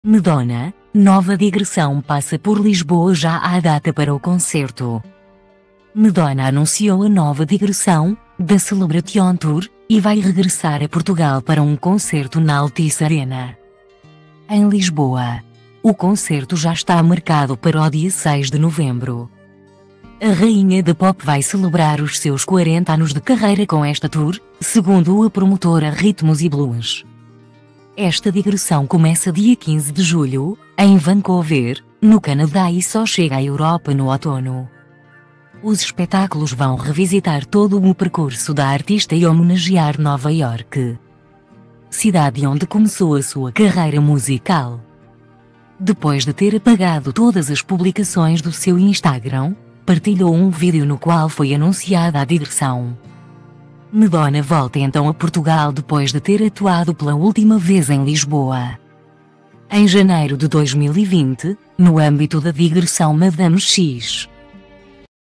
Madonna: Nova digressão passa por Lisboa já há data para o concerto
Madonna anunciou a nova digressão, The Celebration Tour, e vai regressar a Portugal para um concerto na Altice Arena,em Lisboa. O concerto já está marcado para o dia 6 de novembro. A rainha da Pop vai celebrar os seus 40 anos de carreira com esta tour, segundo a promotora Ritmos e Blues.